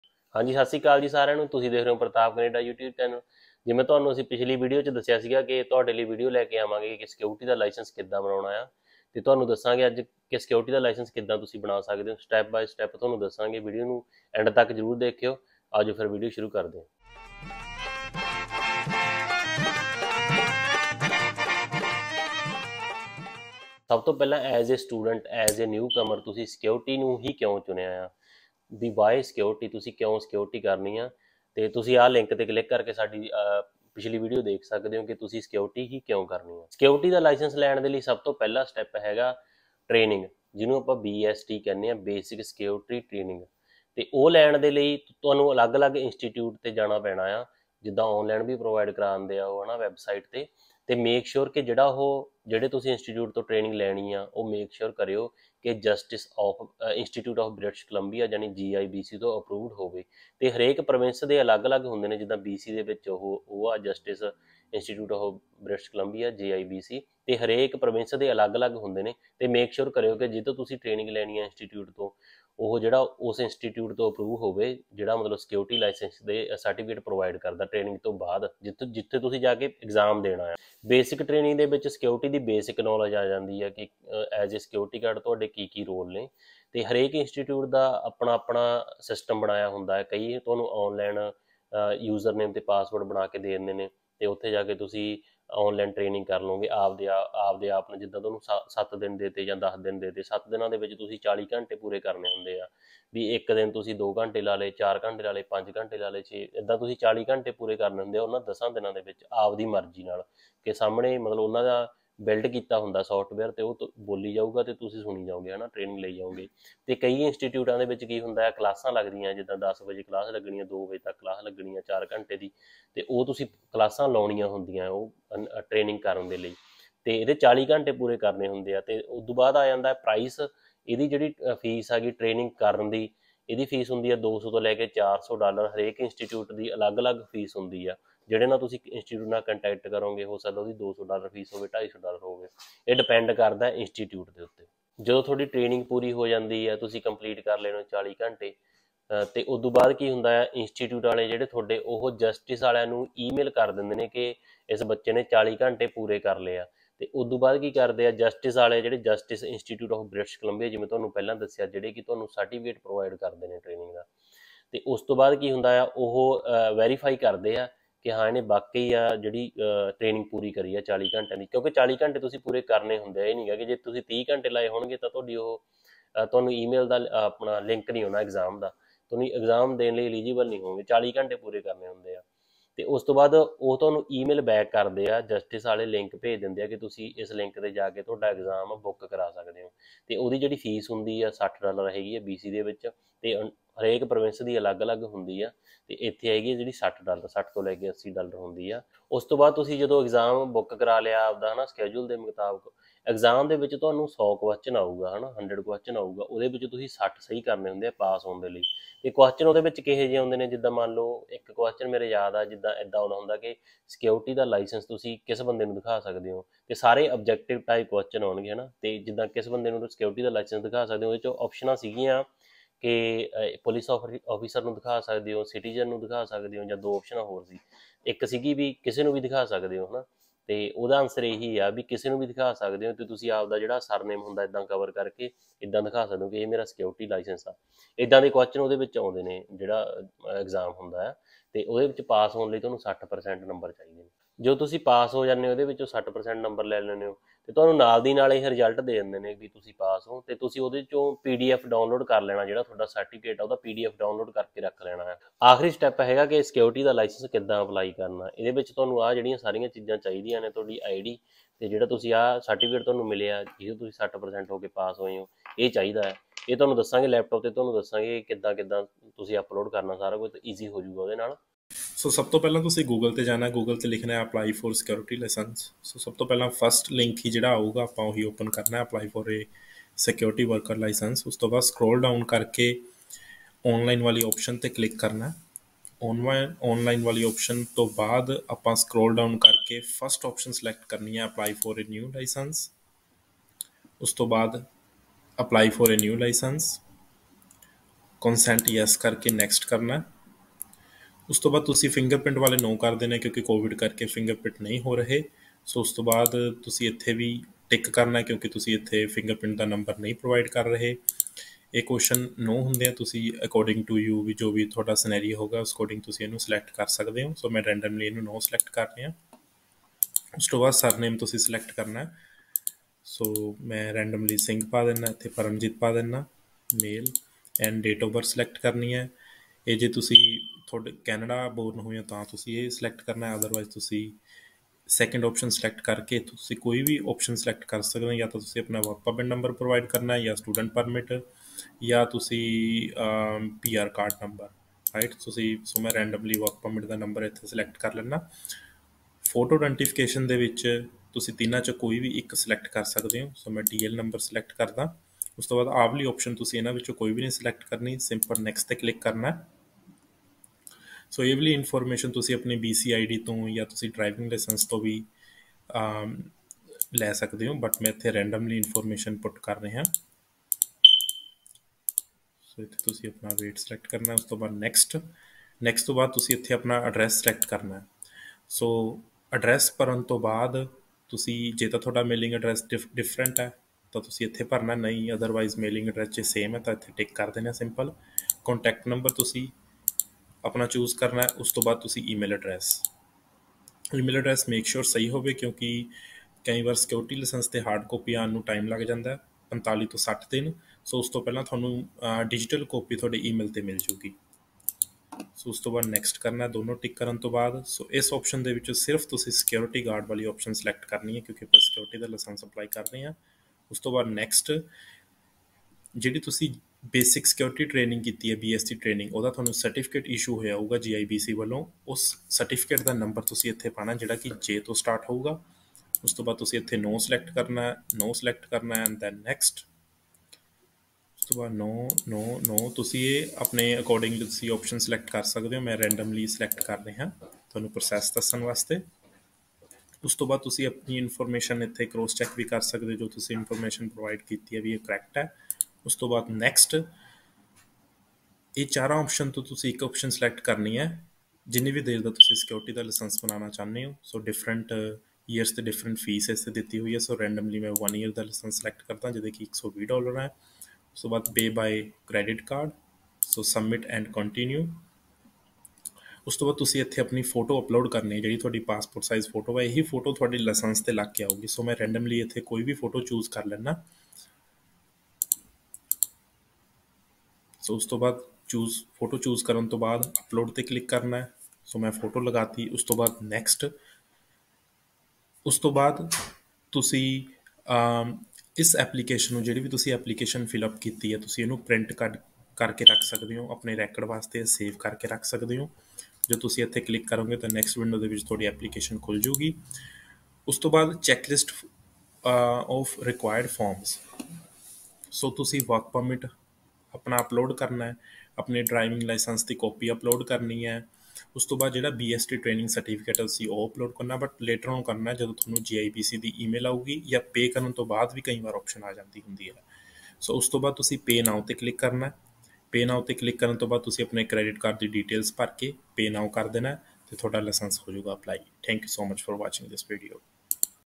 How to get Security License In Canada 2022 (British Columbia) || Fully Explained
HIT US WITH SUPER THANKS IF THIS VIDEO HELPS TO YOU

Steps to get Security License in Canada in 2022 . Full Process